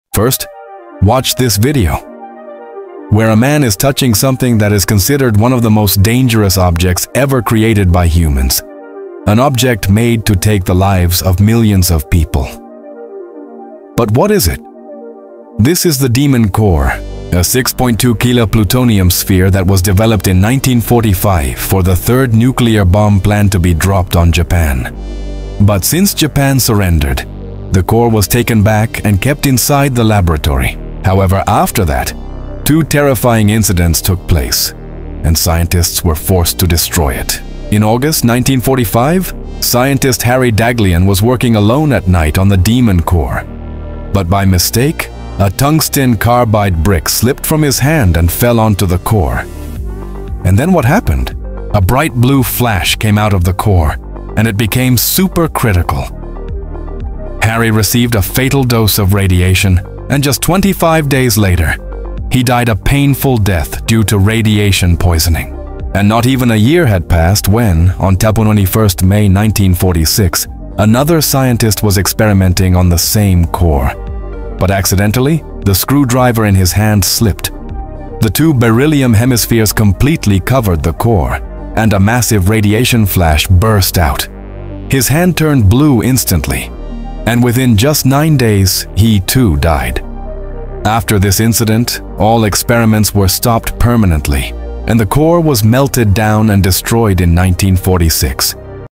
“The Demon Core 💀”
The Demon Core was one of the most dangerous objects ever created by humans — a 6.2 kg plutonium sphere originally built for a third nuclear bomb in 1945. After Japan surrendered, the core was brought back to the lab… where two tragic accidents changed history forever.

Scientist Harry Daghlian died after a blinding blue flash exposed him to a fatal radiation dose. Less than a year later, another scientist lost his life when a screwdriver slipped, triggering a massive radiation burst.

These back-to-back tragedies forced scientists to stop all criticality experiments and destroy the Demon Core permanently in 1946.

This is the terrifying true story of a silent killer that claimed lives — without ever being used in war.

💀 Watch till the end for the full emotional story.